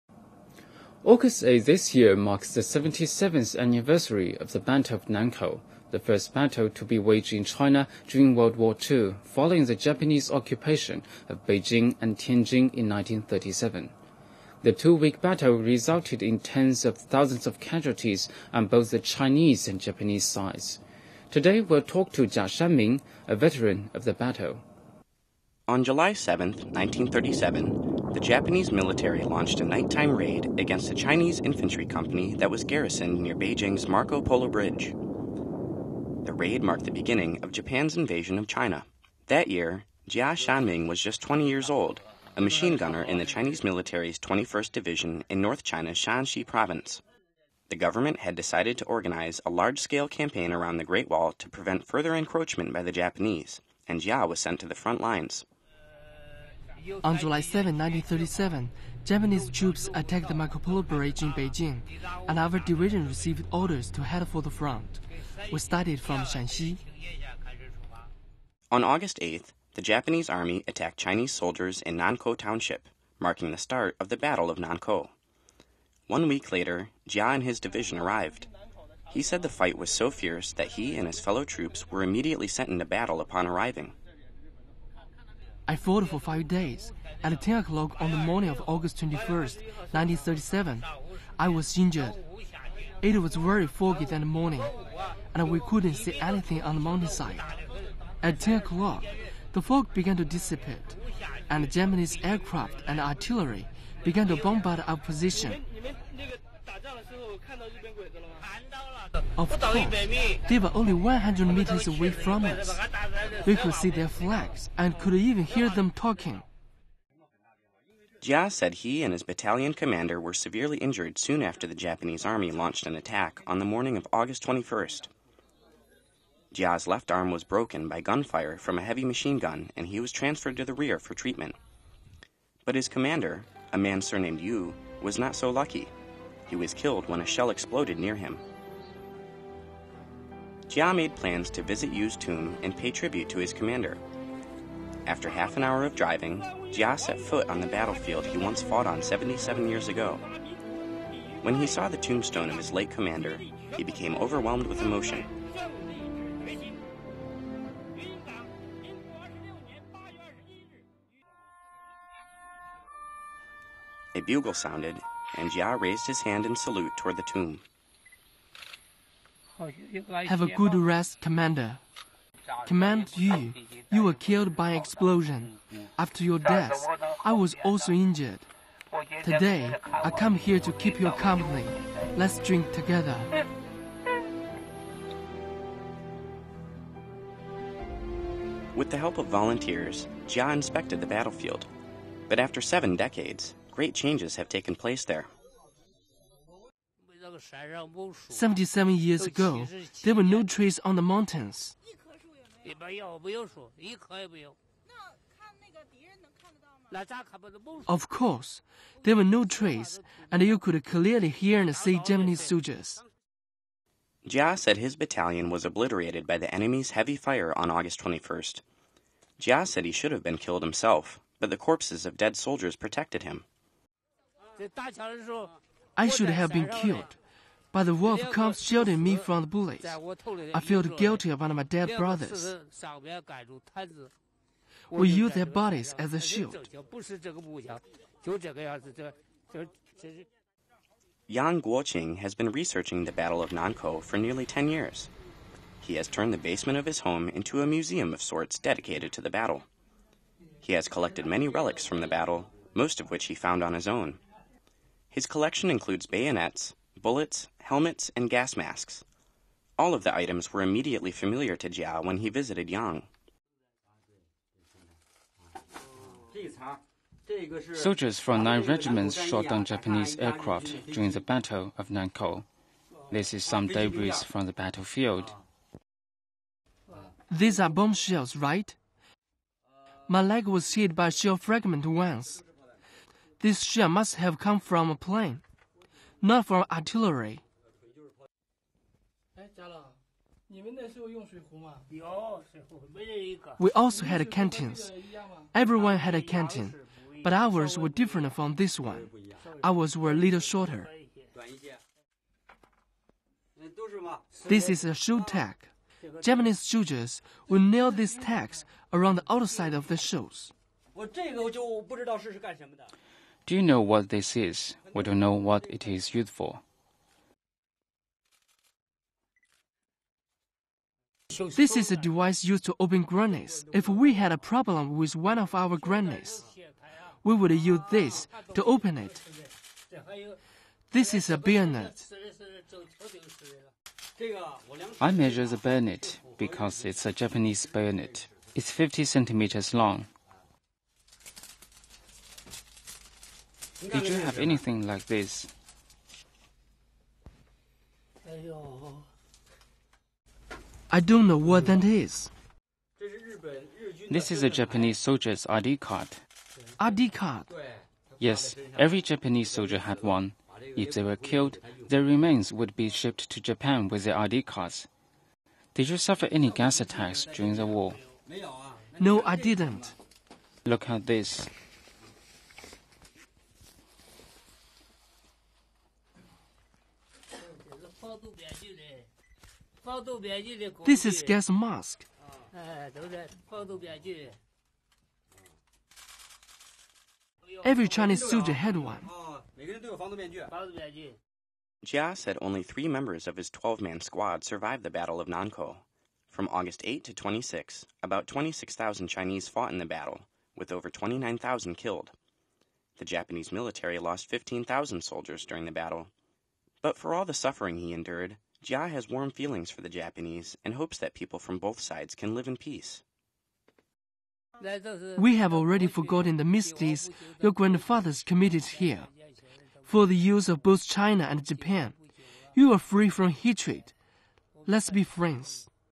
Chinese WWII veteran recalls battle of heavy casualties
A 97-year-old Chinese vetern recalls the fierce Battle of Nankou, the first battle to be waged in China during World War II following the Japanese occupation of Beijing and Tianjin in 1937, ahead of the 77th anniversary of the battle on Aug. 8.